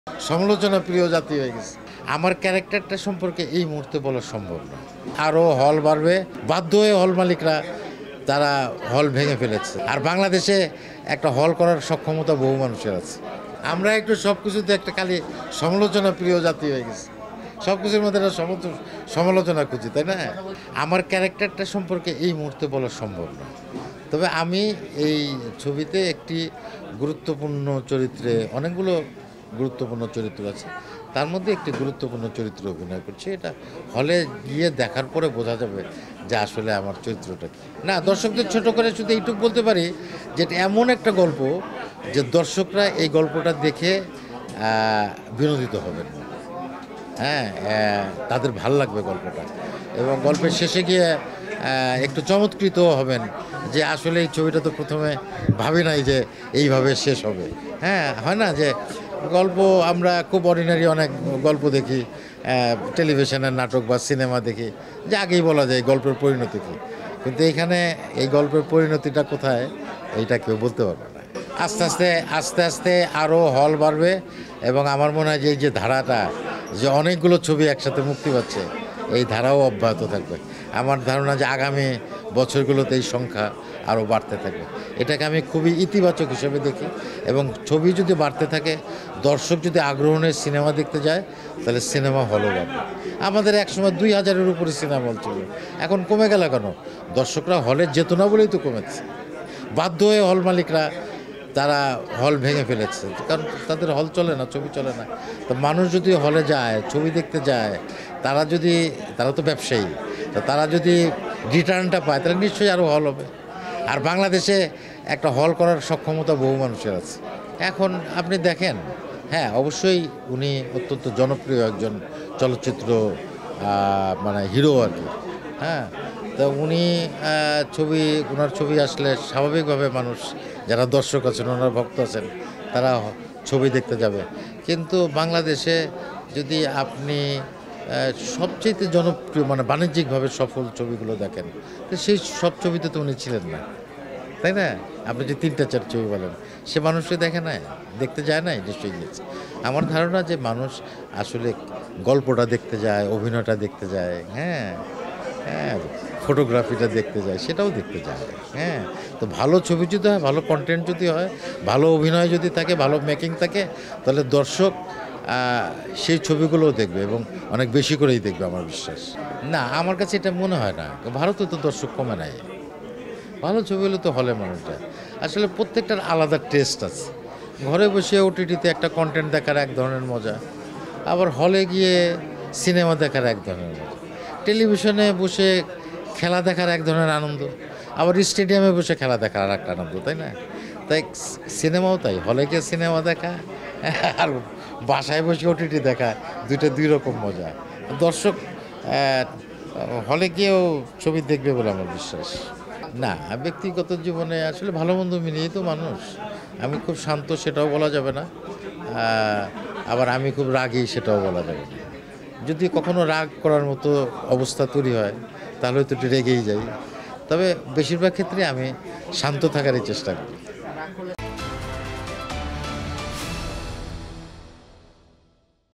আমরা সমালোচনা প্রিয় জাতি হয়ে গেছি: ফজলুর রহমান বাবু | 'ওমর' | Entertainment News | Desh TVaa
42, Shaheed Sangbadik Selina Parveen Sarak, Malibag 1217 Dhaka, Dhaka Division, Bangladesh